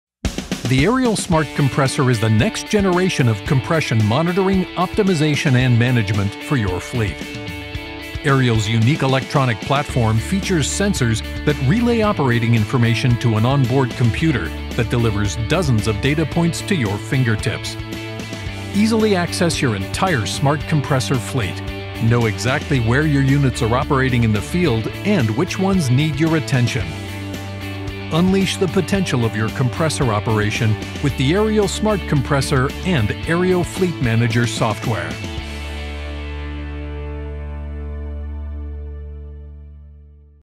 The Ariel Smart Compressor
The Ariel Smart Compressor is the next generation in compression monitoring, control, diagnostics, optimization, and management.